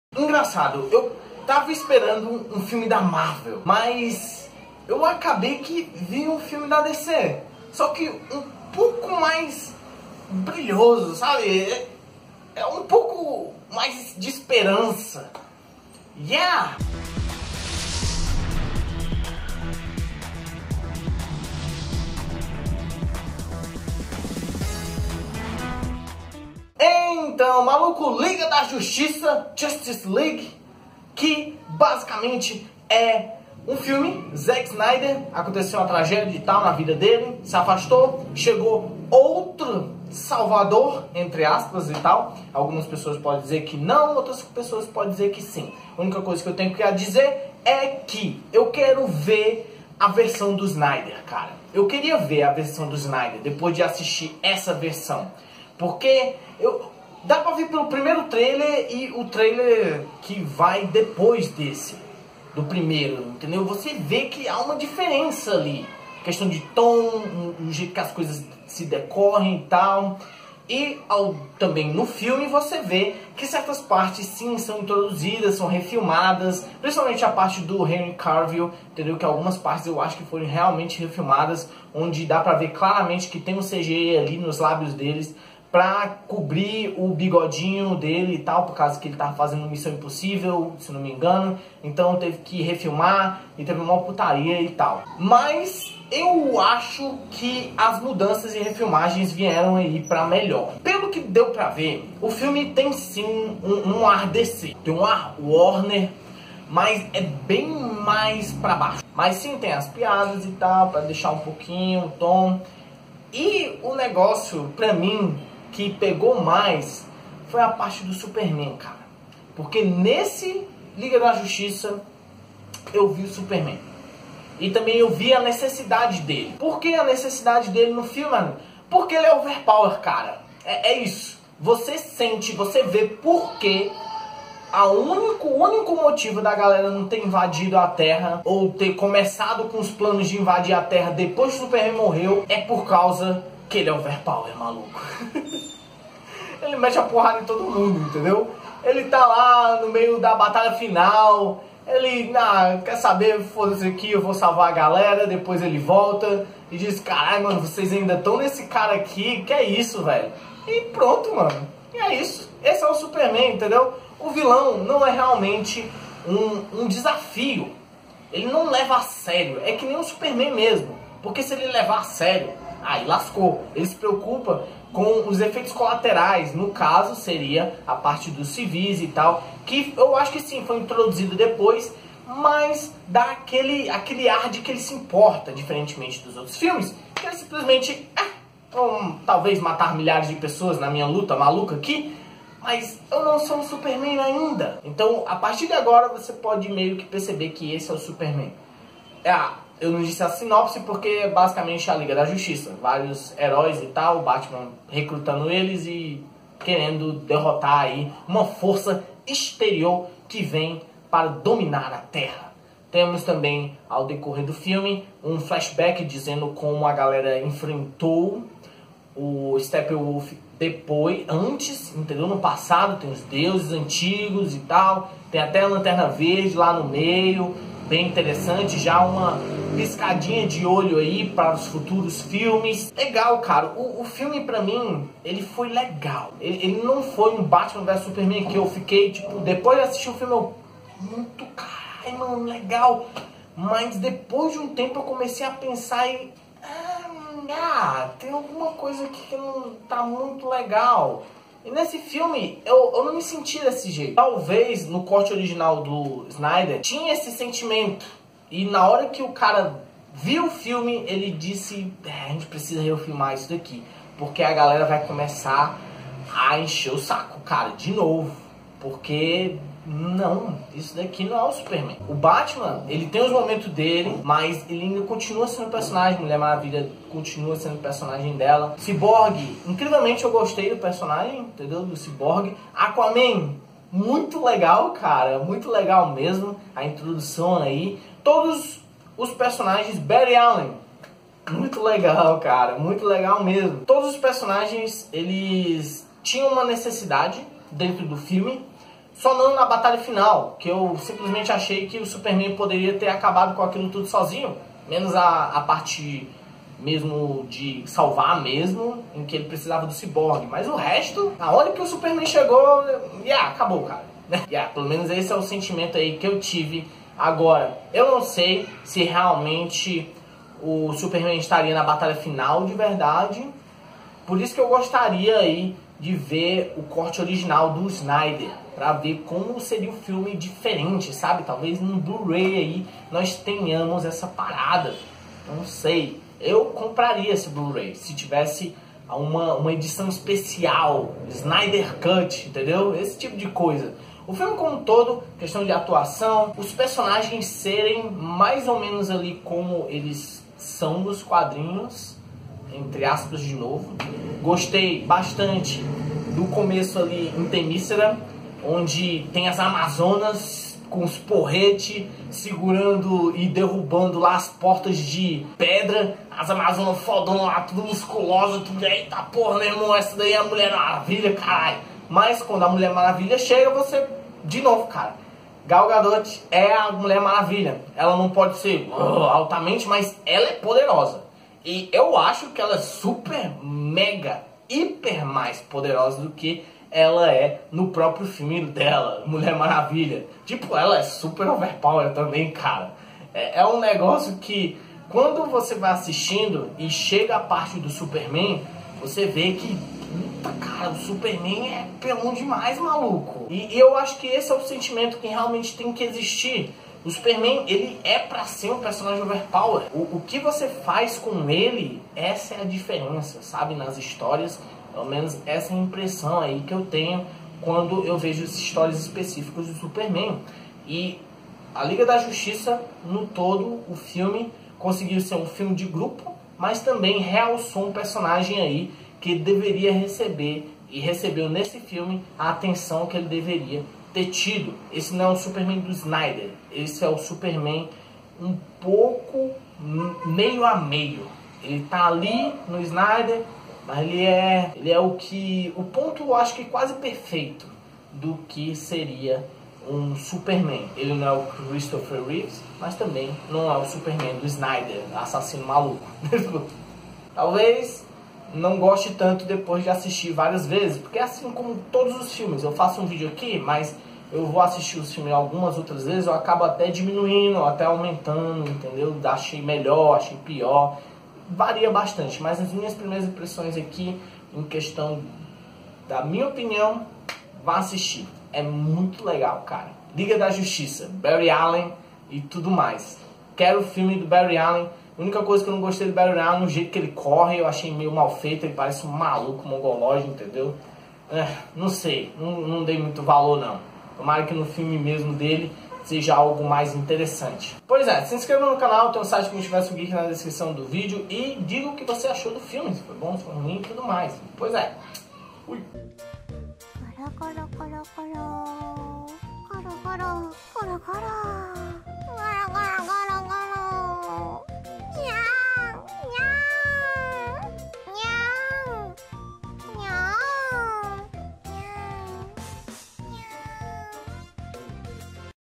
Liga da Justiça | Crítica | Vale a Pena Assistir?
Liga da Justiça 2017, uma crítica com basicamente nada de spoilers... o filme não tem twist nem reviravolta então nem spoiler o negocio tem de verdade superman sendo superman finalmente snyder fora = felicidade.